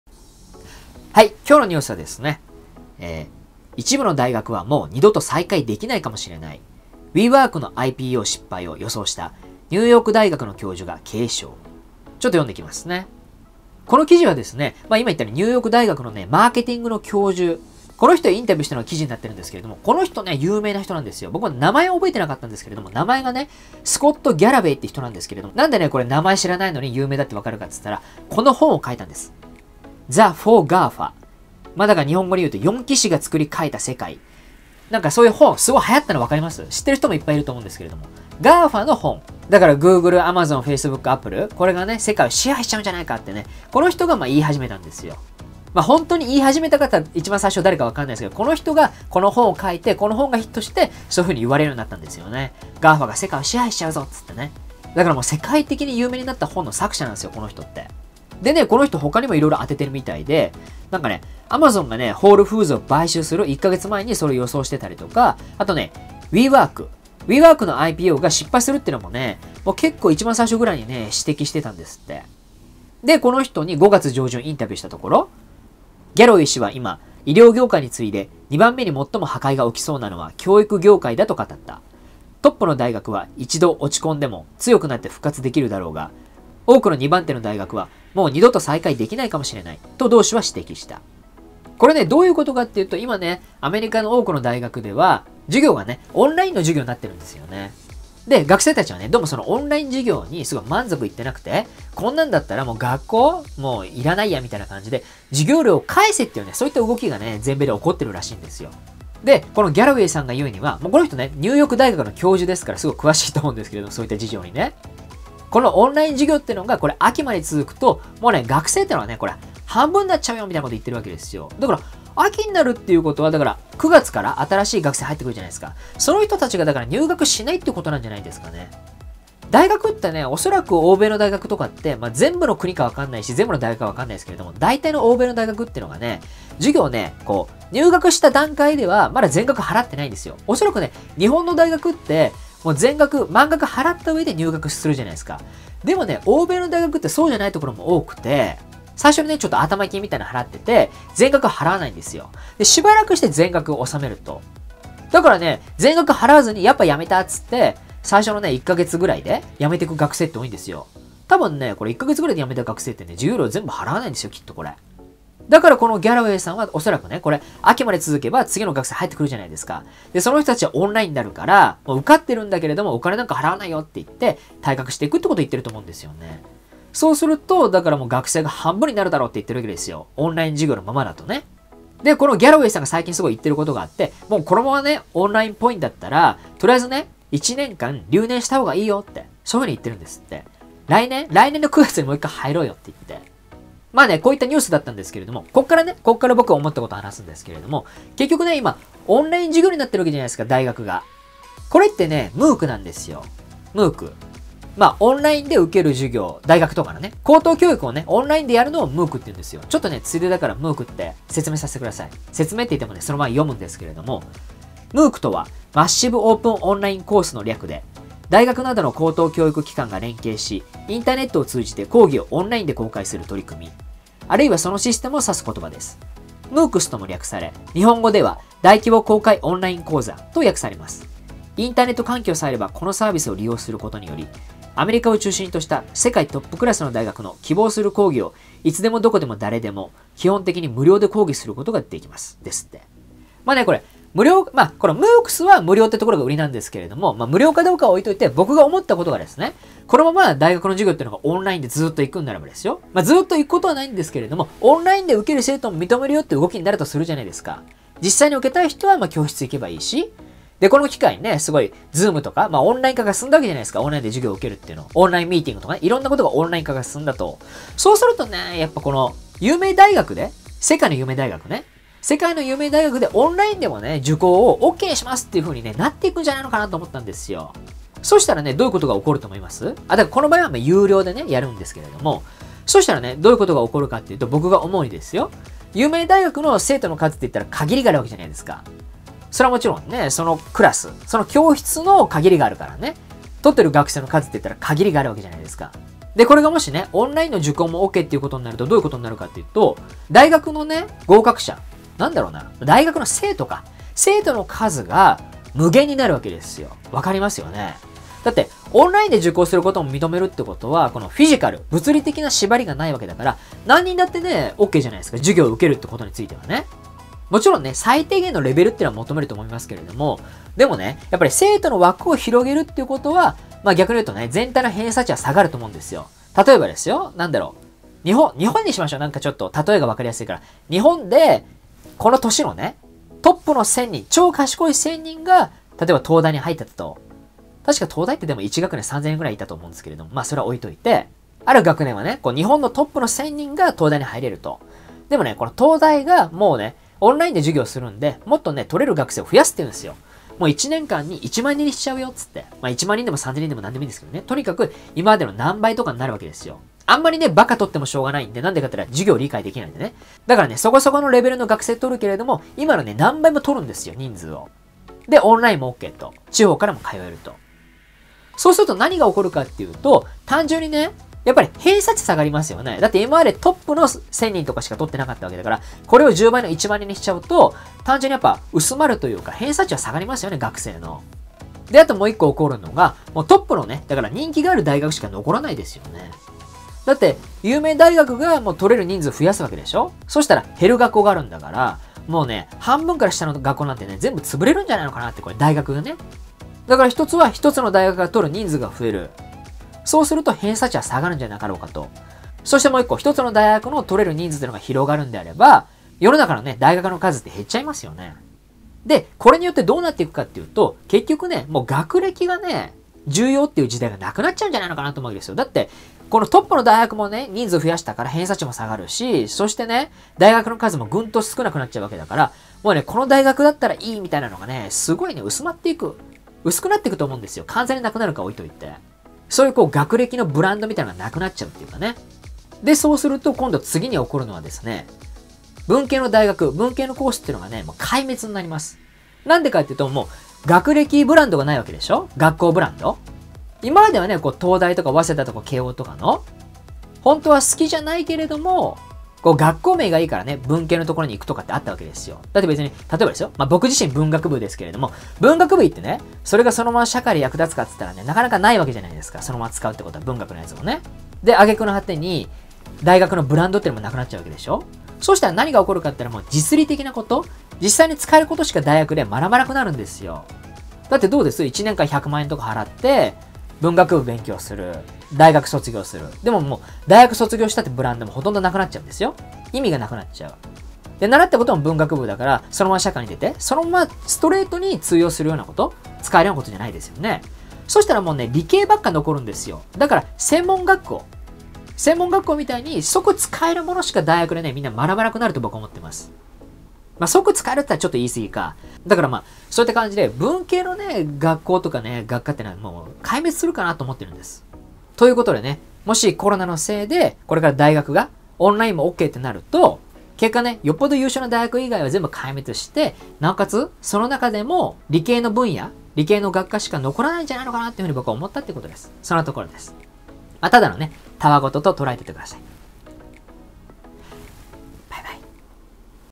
2020年アフターコロナは大学が半分以上消えます 中小企業倒産ラッシュ これから世界どうなるのか【世界経済崩壊と新しい世界が始まる】
暇つぶしのための雑談動画チャンネルです。話している内容など確認などしていませんので鵜呑みにしてはいけません。

■ライブ配信告知メインのLINE＠：IDは「@595dsidc」

〒153-0042
株式会社Kiii TTMつよし 宛